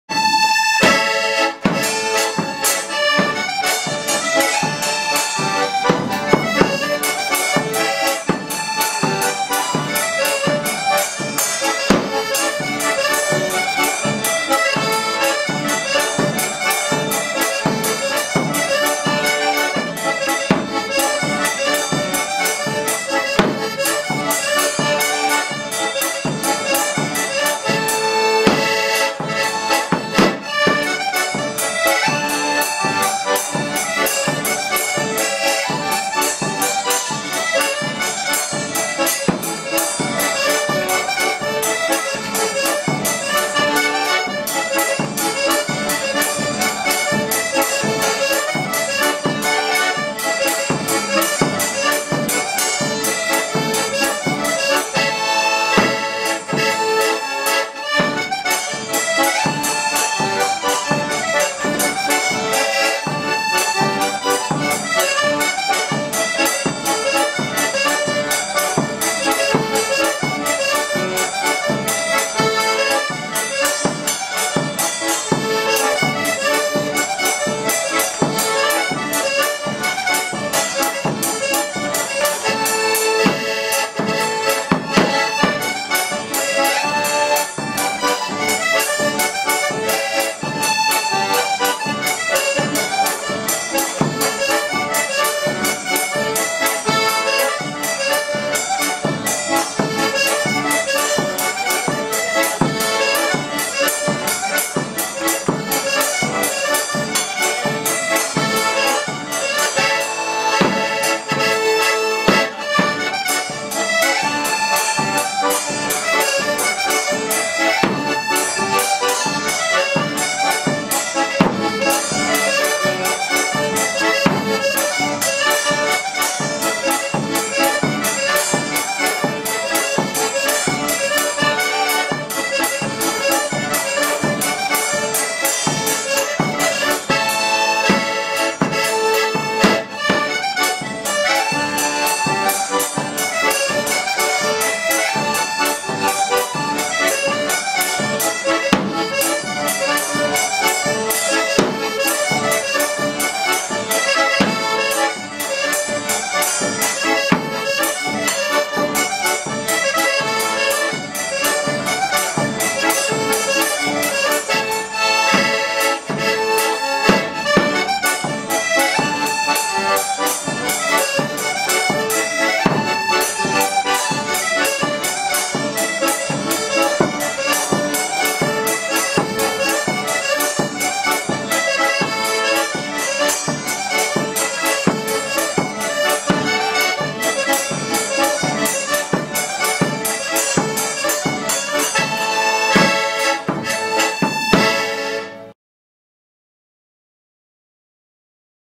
2022 03 20 Smogorzów 13
Grają: Sylwester Kuc – akordeon Soprani Castello, Andrzej Mandecki – baraban z talerzem obaj ze Smogorzowa.